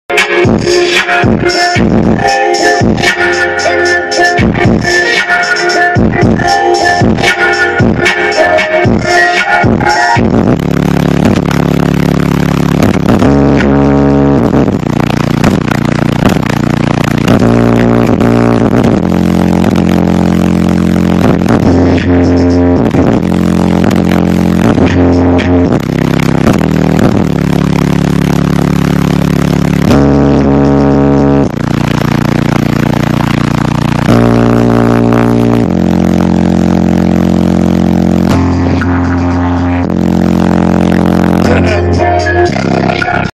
Hey girl, you like bass? Come check this out!
Demo at Rock Springs Rods & Rails Car Show 2013
Thomas Obrien posted a 155.9 on the Term-Lab db Drag before the demo.